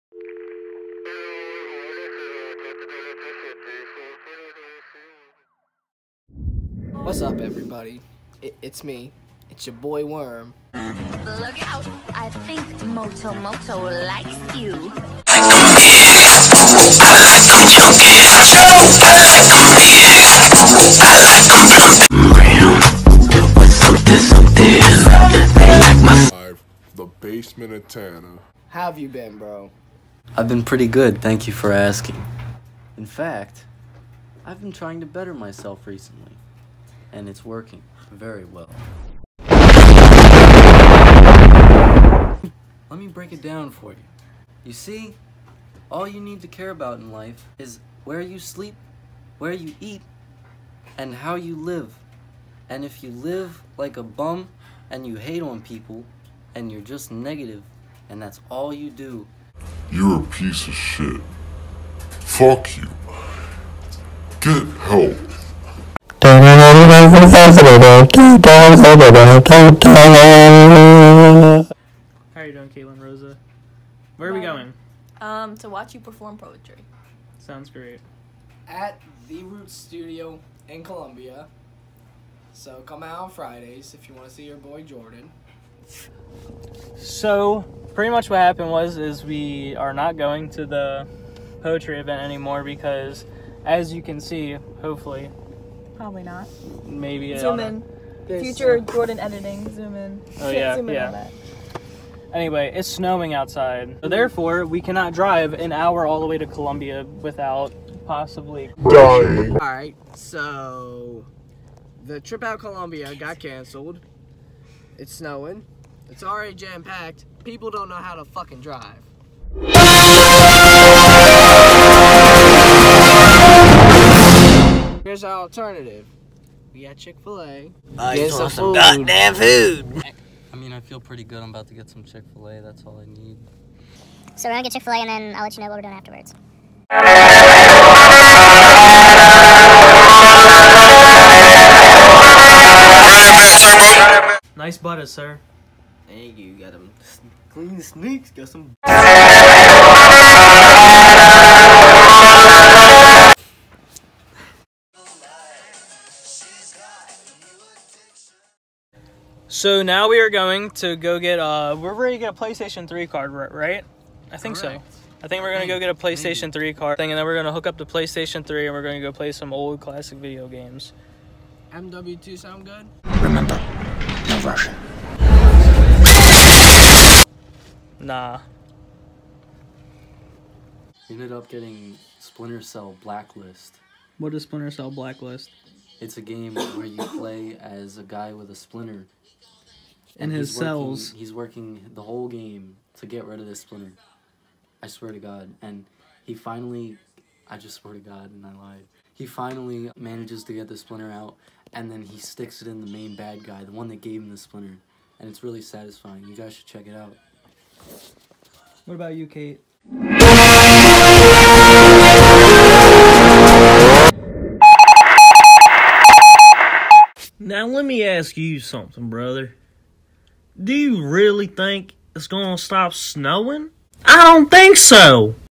The Snow Ruins Everything
The snow ruins our open mic night chances... so we take a trip to chic fi la!